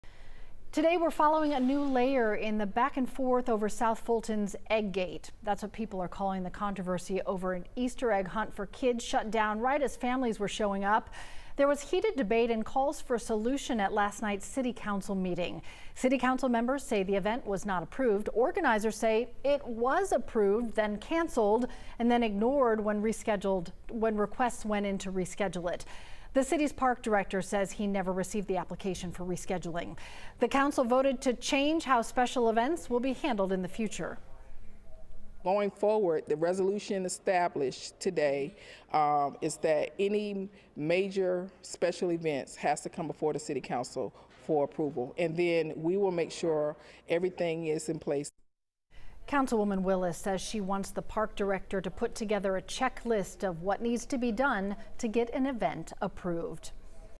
South Fulton adopts new special events resolution after Easter egg hunt shut down
What some people in the community have called "Egg Gate" was at the center of a fiery meeting Tuesday.